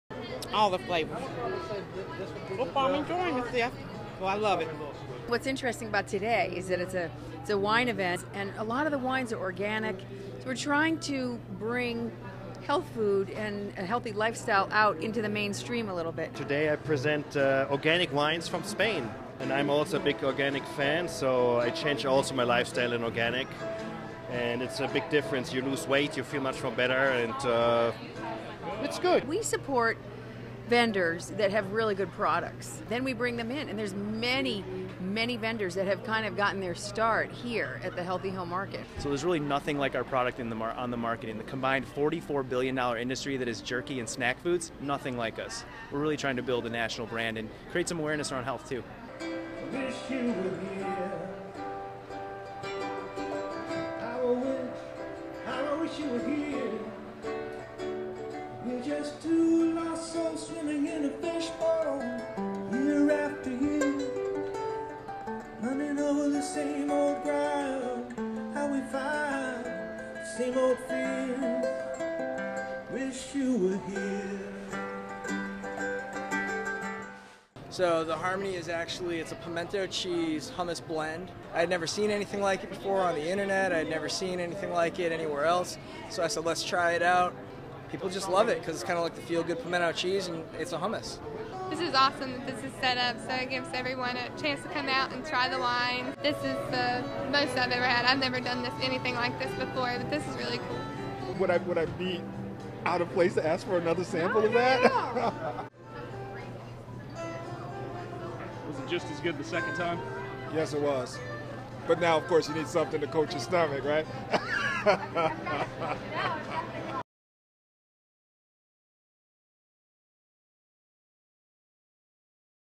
Jr. Digital Journalist Logan Sherrill - Unwind With Wine
Healthy Home Market in Charlotte was the hot spot for wine-o-holics today. The supermarket saw an increase in traffic with people passing through to try the latest wines & beers from both local and national vendors.